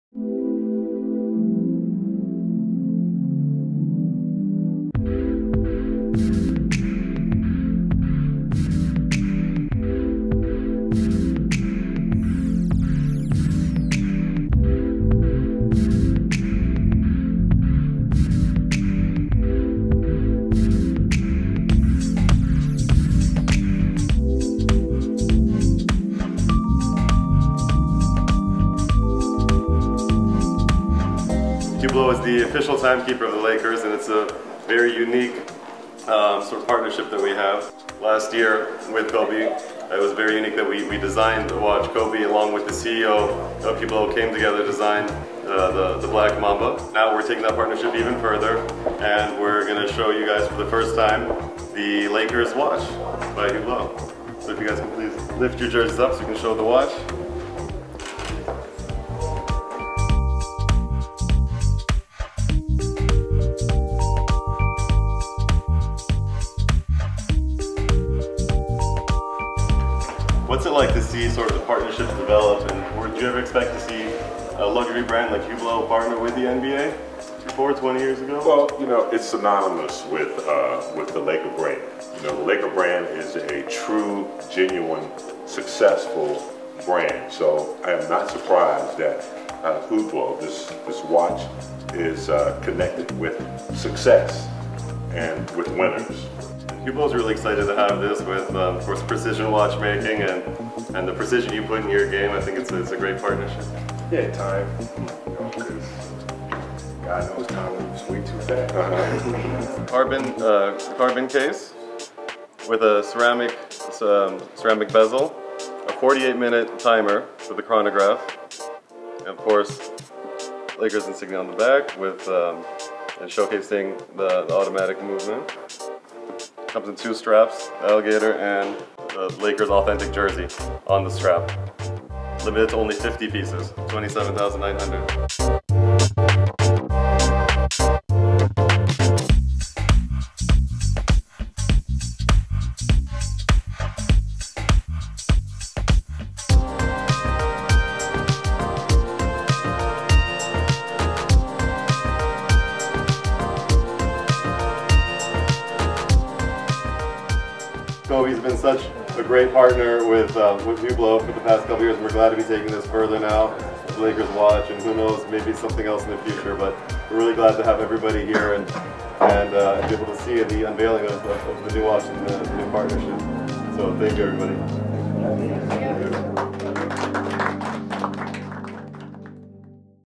SLAM DUNK WITH THE LEGENDARY LAKERS!!
Lakers star player and Hublot Brand Ambassador, Kobe Bryant, joined in the celebration with Hublot Partners John and Greg Simonian to unveil the new timepiece to the public for the first time.

Hublot, the art of Fusion concept in Watchmaking, combining exotic materials in Swiss watches. Discover the world of Hublot on: